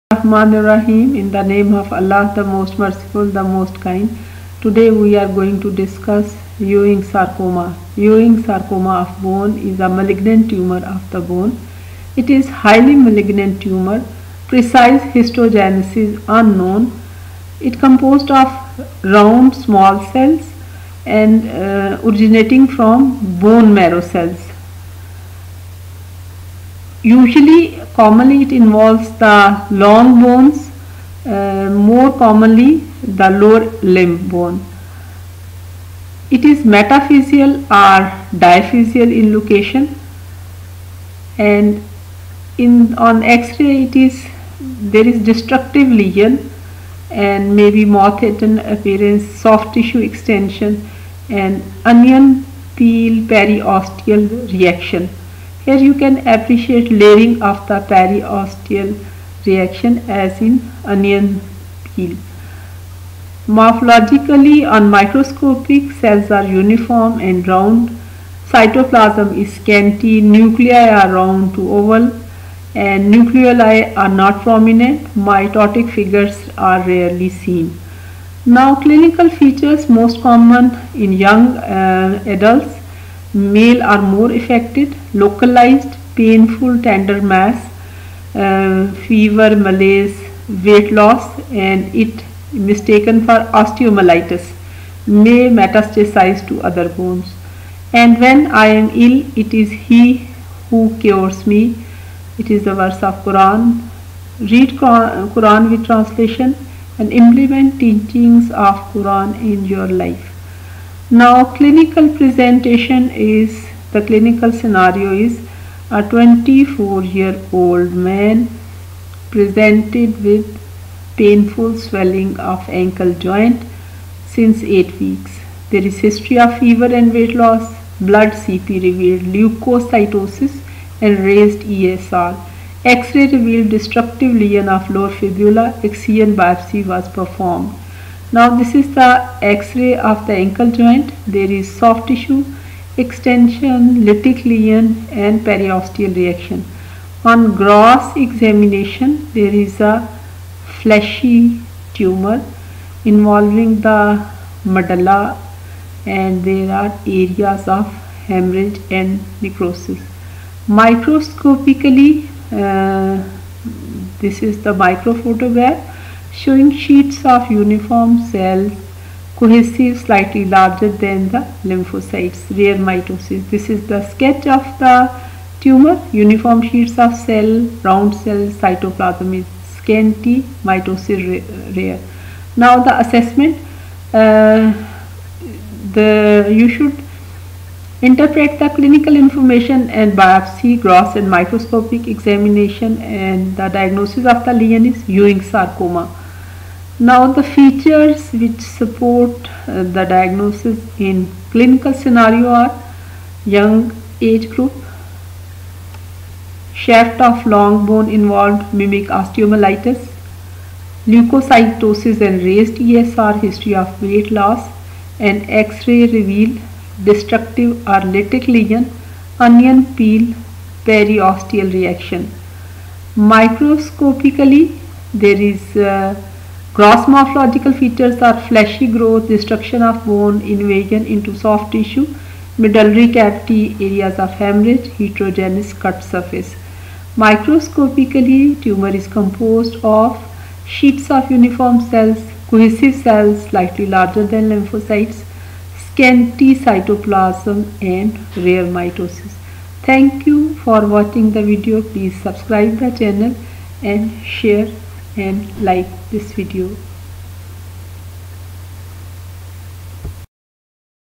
Ewing sarcoma skill lab
Ewing sarcoma, clinical presentation and morphology for medical students